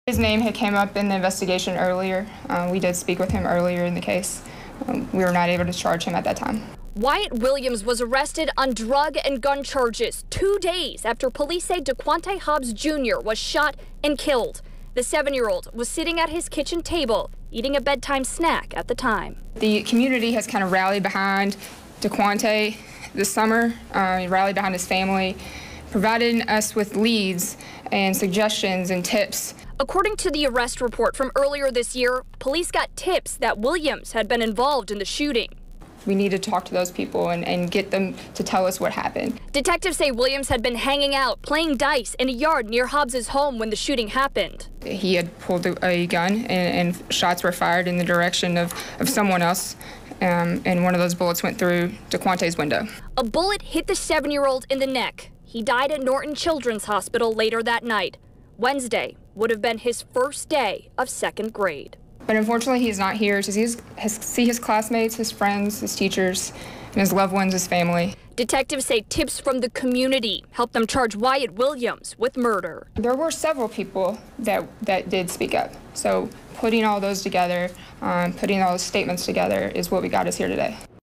Suspect charged in murder of 7-year-old boy
Wyatt Williams, 23, is charged with shooting and killing Dequante Hobbs Jr., 7, in May.